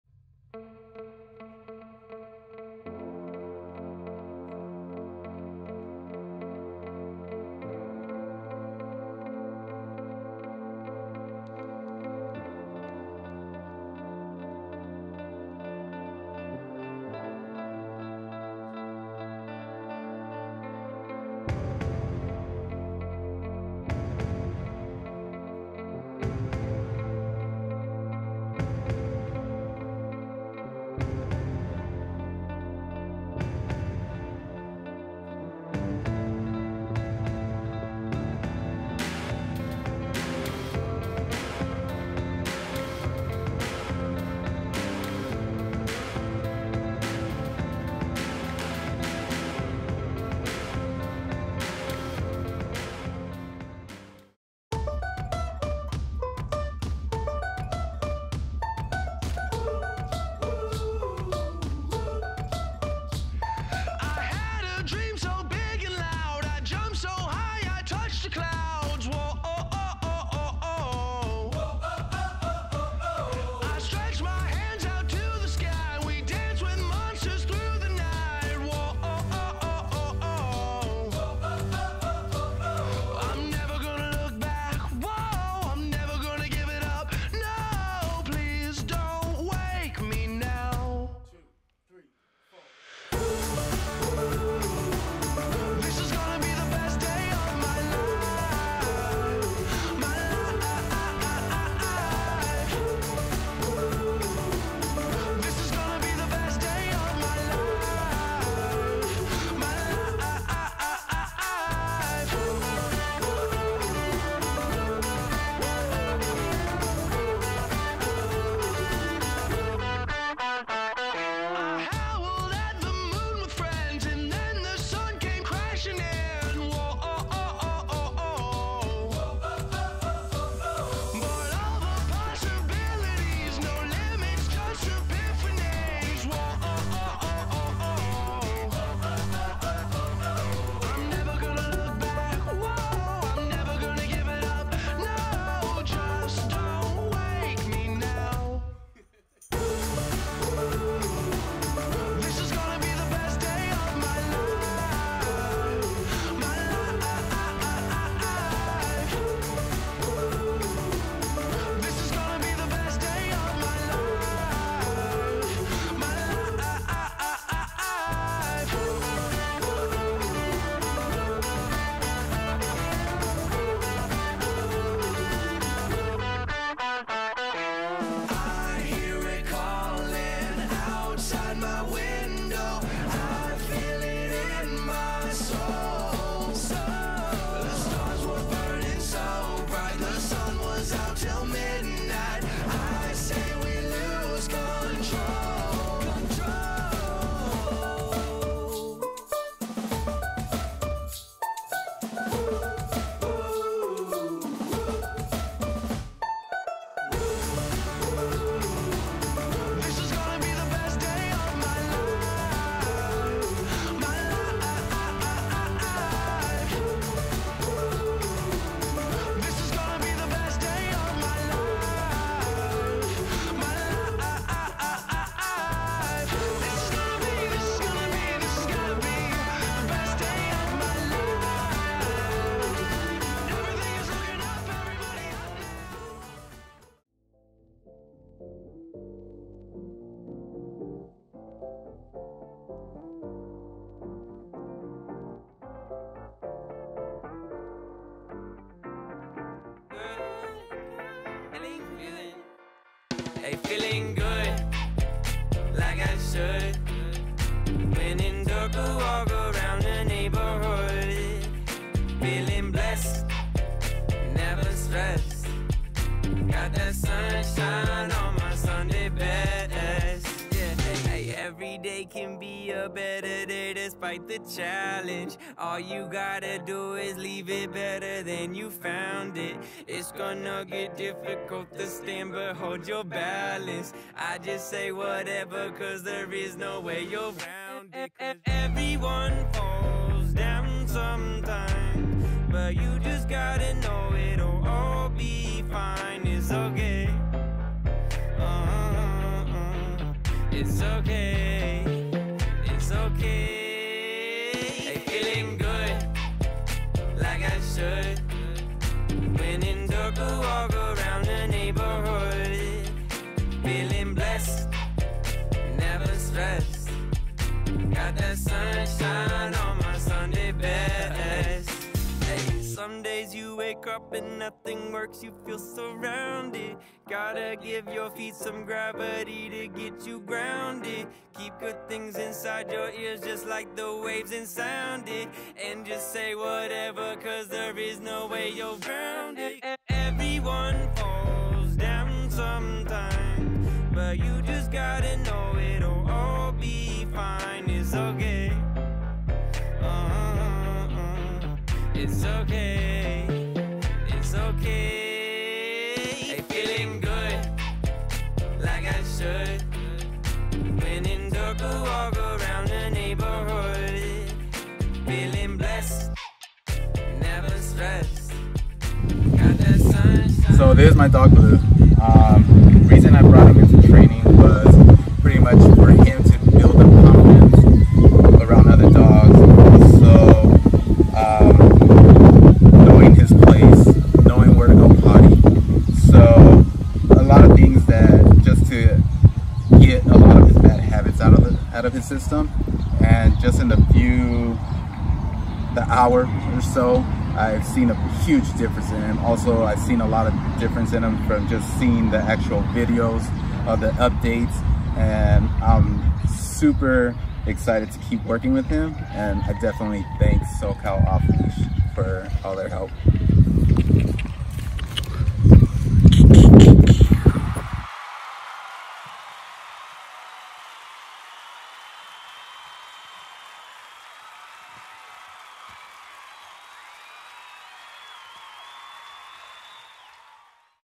French Bulldog Training | Blue | Norwalk, CA
Blue, a French Bulldog from Norwalk, CA went through the Balanced Canine Training SoCal Board and Train Program. Blue was trained by Chris Louie in Norwalk, CA.

Blue came to Balanced Canine Training SoCal with varying behavioral issues, including pulling on the leash, squirrel chasing, jumping up when greeting, counter surfing, generally not listening to commands, and barking at strangers. Balanced Canine Training SoCal was successful in transforming Walker into a well-managed pup.